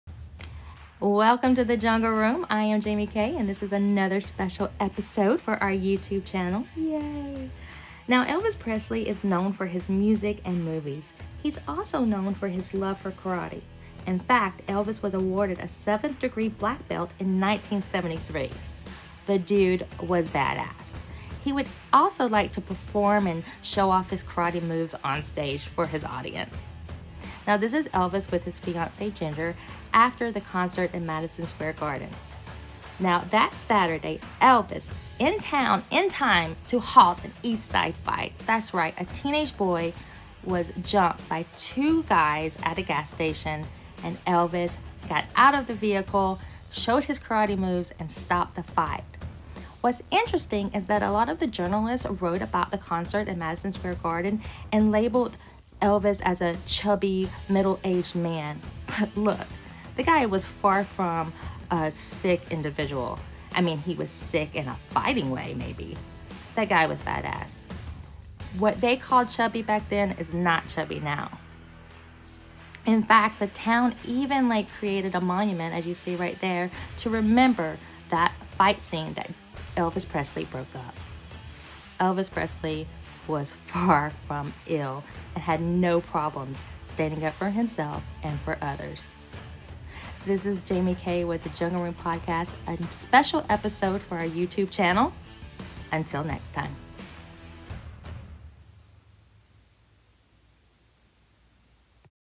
Elvis Presley: Karate Hero
Elvis breaks up a fight after a 17 year old boy gets jumped by two men at a gas station.